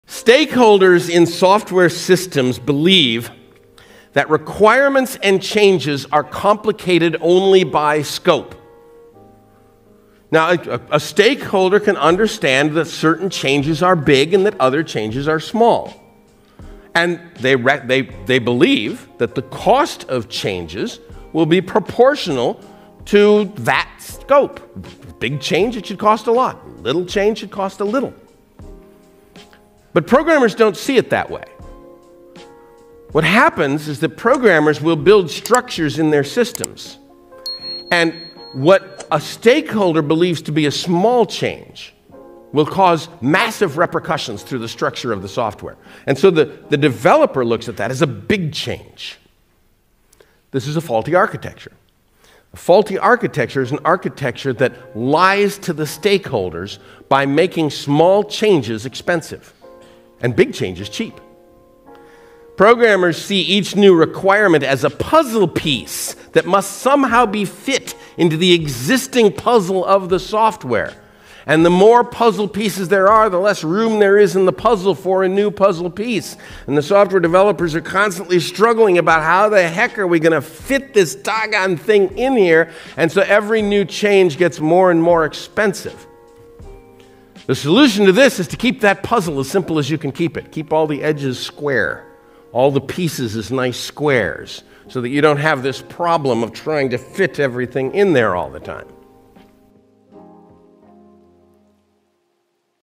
The true cost of a software change - Uncle Bob
In tihs video, Robert C. Martin (Uncle Bob), the author of Clean code speaks on the true cost of a software change. He takes into account the perspective of the stakeholders as well as that of the perspective and weighs in on the source of the discrepancy between the time the stakeholder expects a feature to be complete and the time a developer can actually have it done. Thoughts?